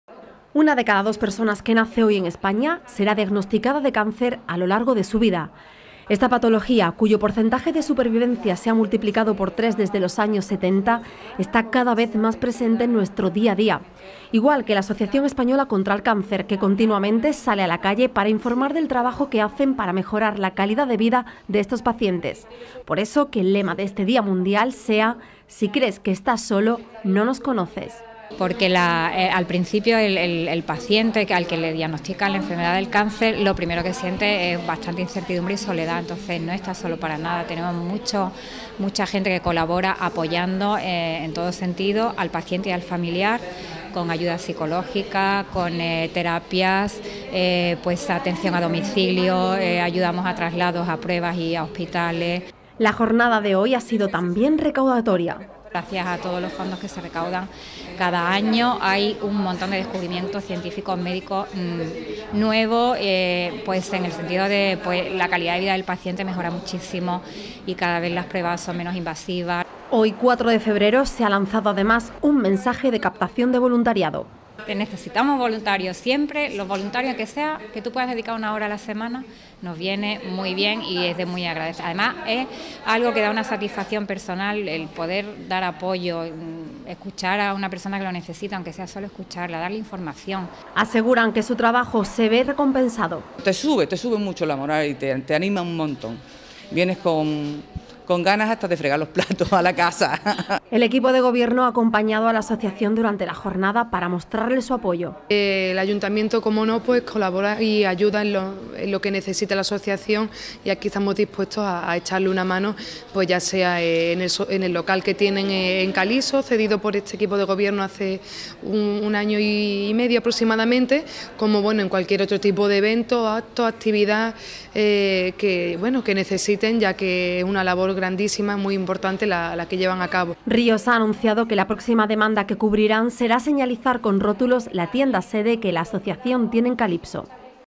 Hoy, 4 de febrero, se celebra el Día Mundial Contra el Cáncer.
Hoy, 4 de febrero, se celebra el Día Mundial Contra el Cáncer. Durante la jornada, los voluntarios de la Asociación Española, que lucha contra esta enfermedad, se han repartido por diferentes puntos del municipio para ofrecer información sobre esta enfermedad y recaudar fondos.